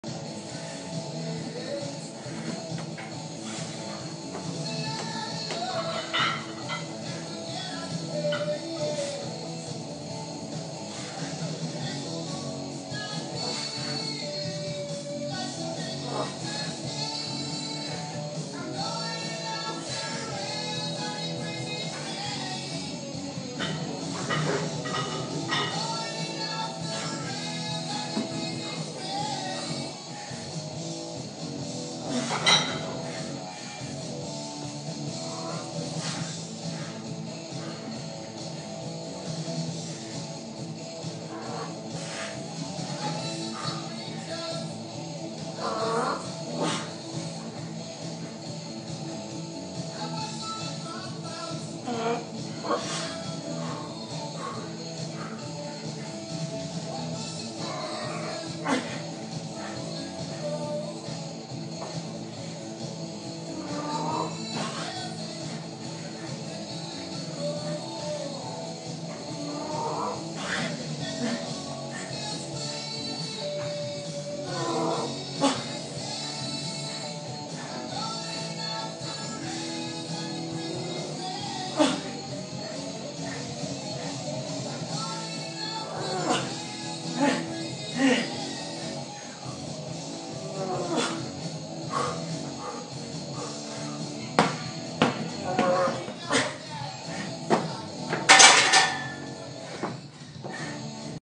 Wayne’s World Front Squats
On March 12, 2015 my brother Bryan and my best friend Mark dared me to front squat body weight for max reps. I rounded up to 205. The after-story of this video is that Brian Oldfield (google him!) saw this and shared it on his Facebook page. He called it good conditioning for throwers.